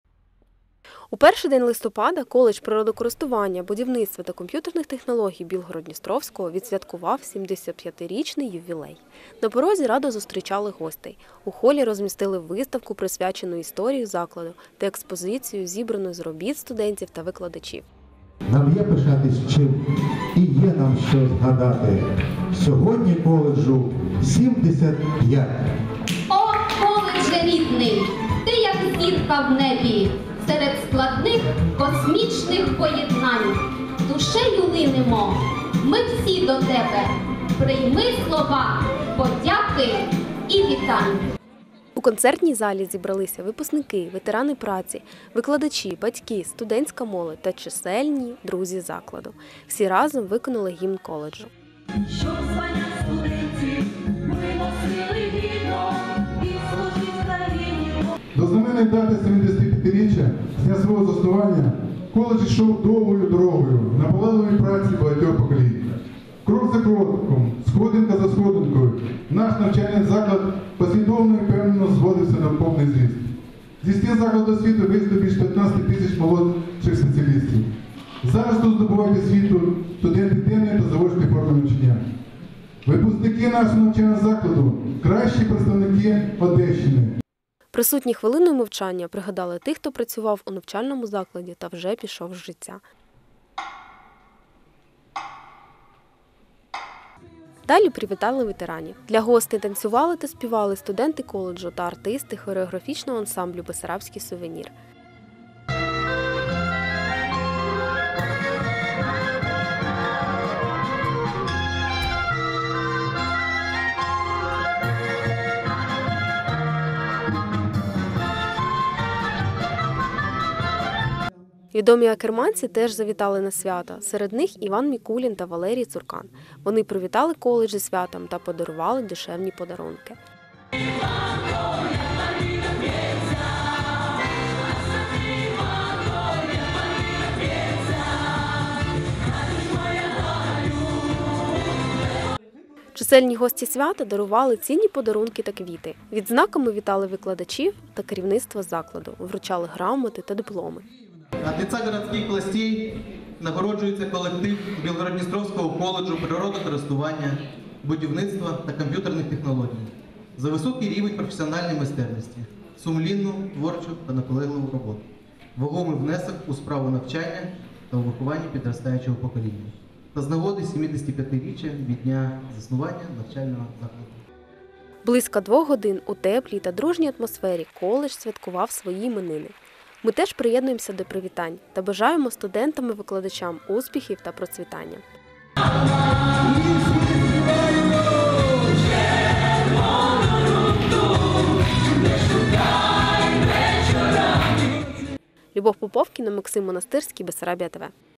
75-річний ювілей коледжу
1 листопада Білгород-Дністровський коледж природокористування, будівництва та комп’ютерних технологій відсвяткував ювілей – поважні 75 років з дня свого заснування. З цього привод у закладі відбувся святковий концерт. Подробиці далі у сюжеті.